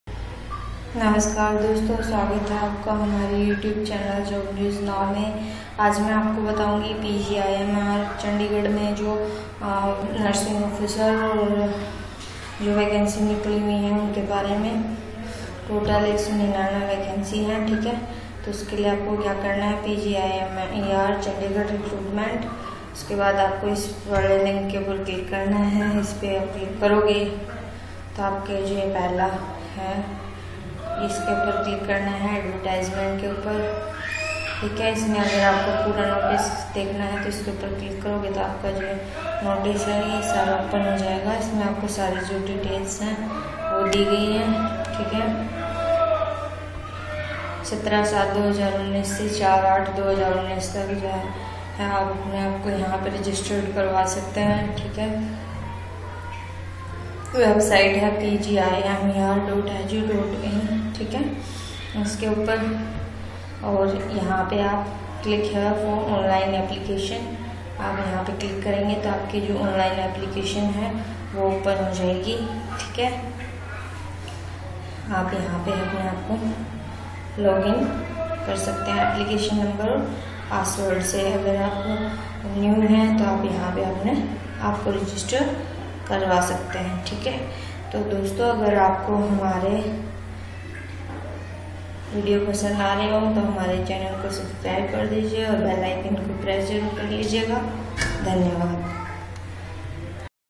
PGIMER CHANDIGARH RECRUITMENT 2019
Hello friends

Postgraduate Institute of Medical Education & Research (PGIMER), Chandigarh
invites online applications from the citizens of India for recruitment of various Group
‘B’ and ‘C’ posts on regular basis for All India Institute of Medical Sciences
(AIIMS), Bathinda (Punjab), as per details given below:-